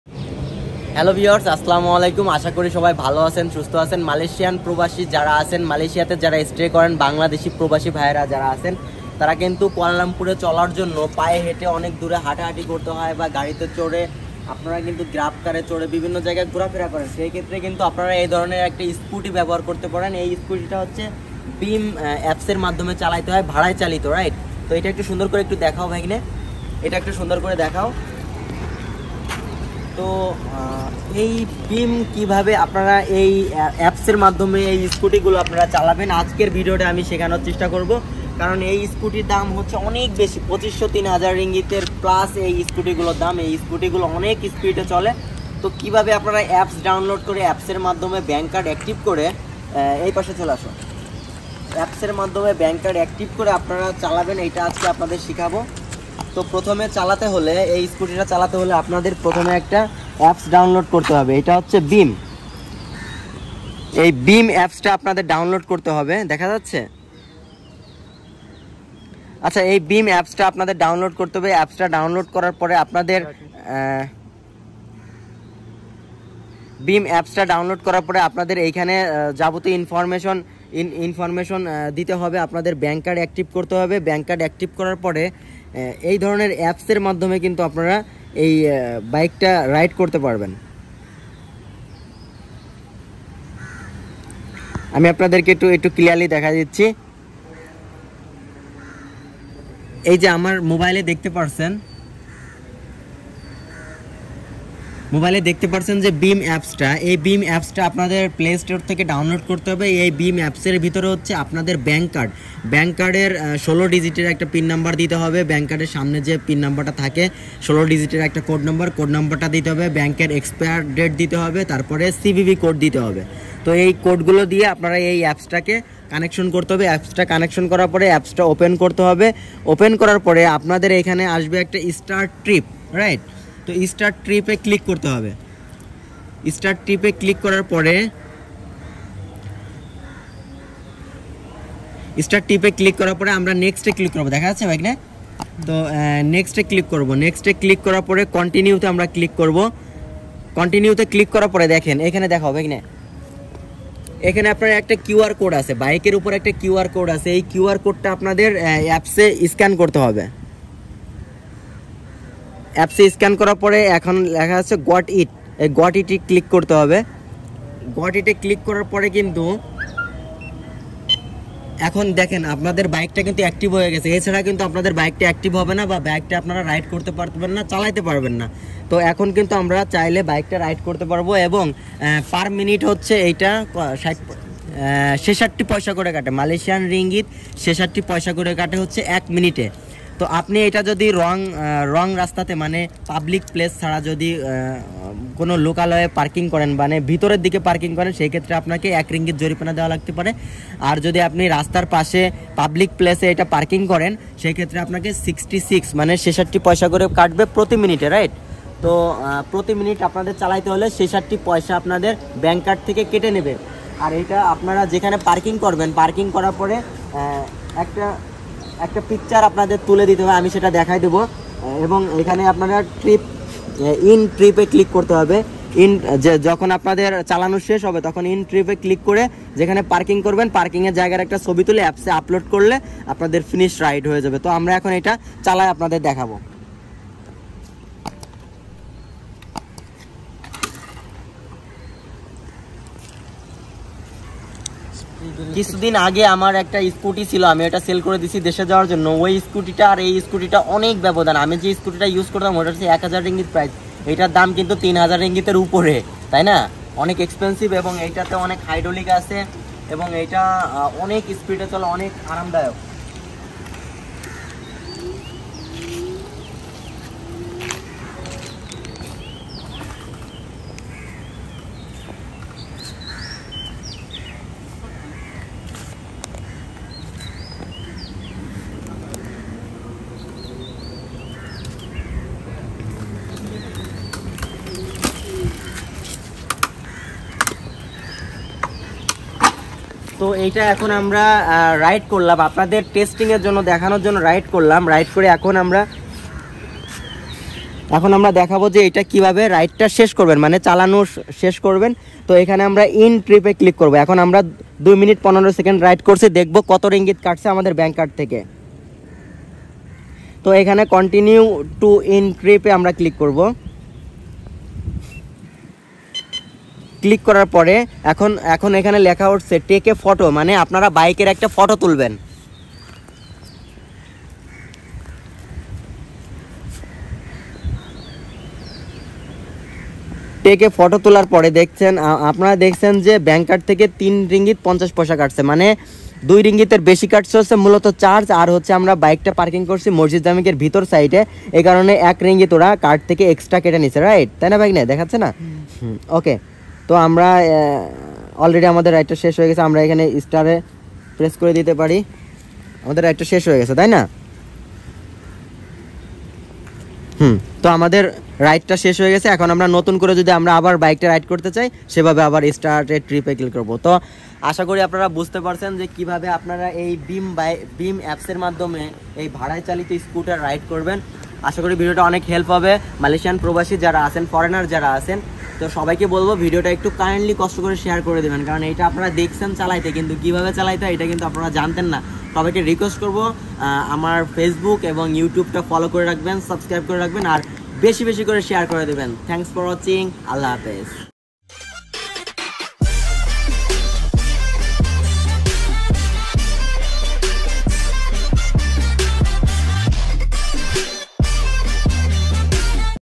মালয়েশিয়াতে ভাড়ায় চালিত স্কুটার কিভাবে চালাবেন বিম এপস দিয়ে।How to ride beam Apps bike in Malaysia
মালয়েশিয়াতে ভাড়ায় চালিত স্কুটার কিভাবে চালাবেন বিম এপস দিয়ে।How to ride beam Apps bike in Malaysia Kualalumpur | @Easin Vlogs